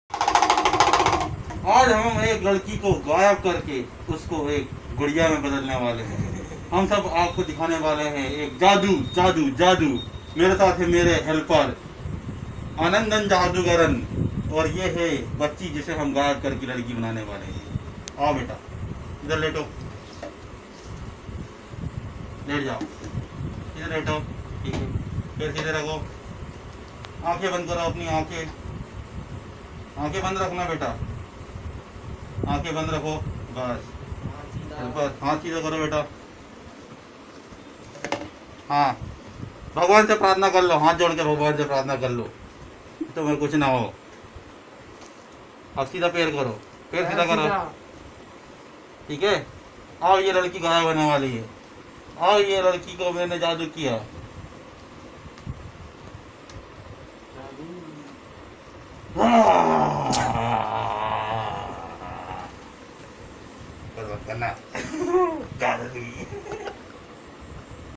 Jaadu ki Pari..
See how a Nepali girl disappears from the crutches of Indian monsters..